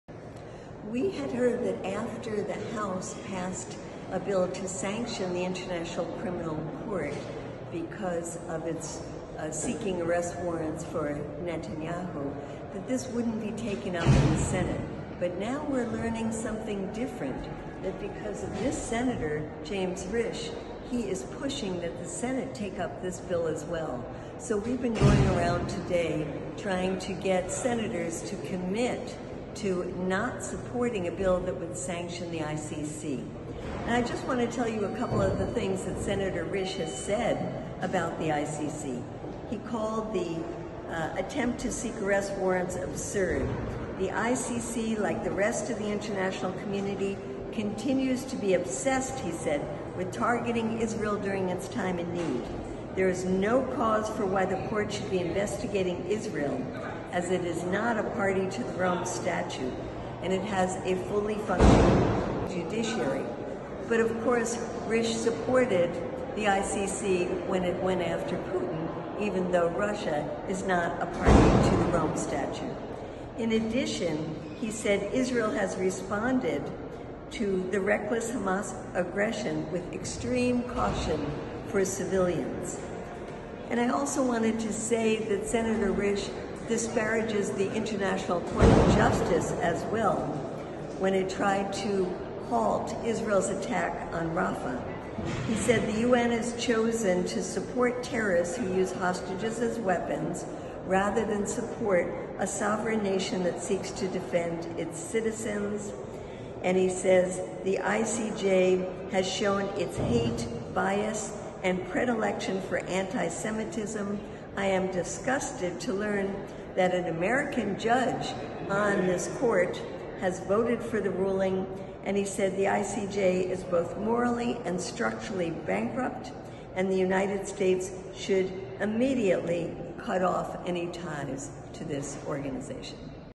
Stop this Senator from sanctioning the ICC!
Opponent of international justice, Senator James Risch is pushing the Senate to sanction the ICC for seeking arrest warrants for Netanyahu.